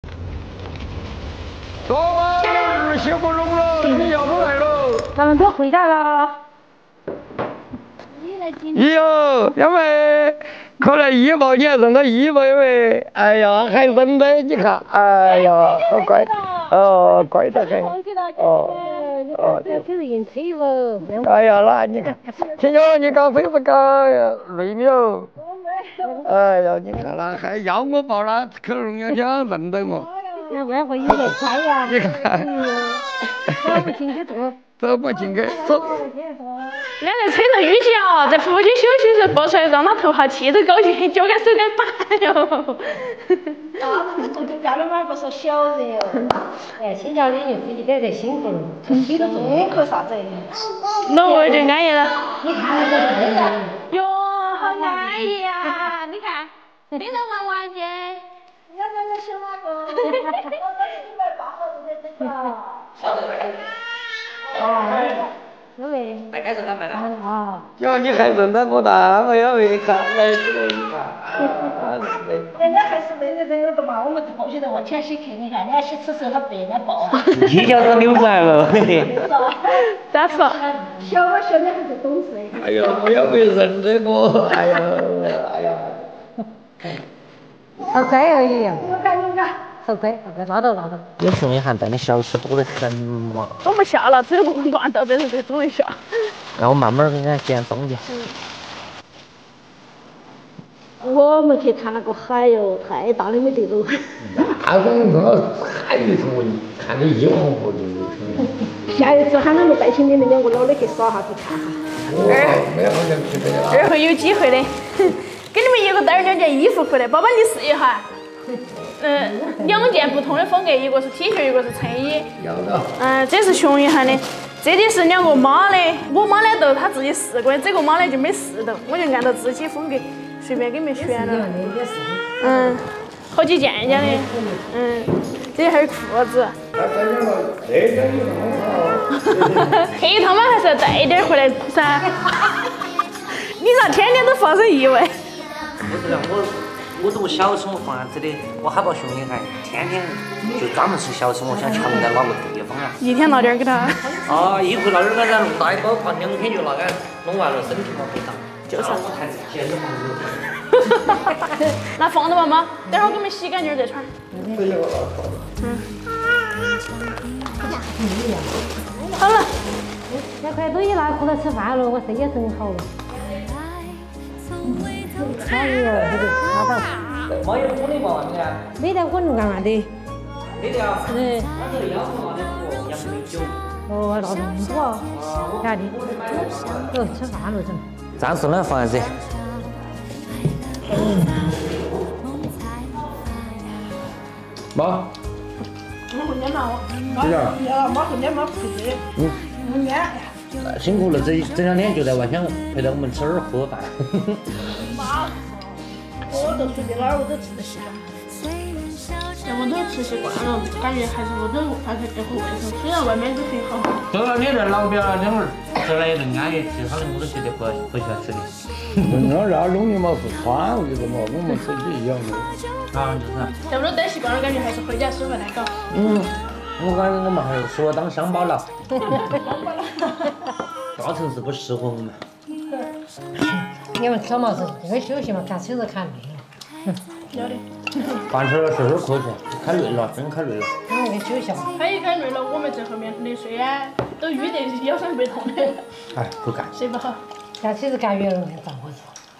我们平安到达四川老家，爸妈做好热腾的饭菜等着，回家的感觉真好
大家好，我是云村姚妹
云南姑娘嫁四川
小两口白手起家
一起努力
分享农村生活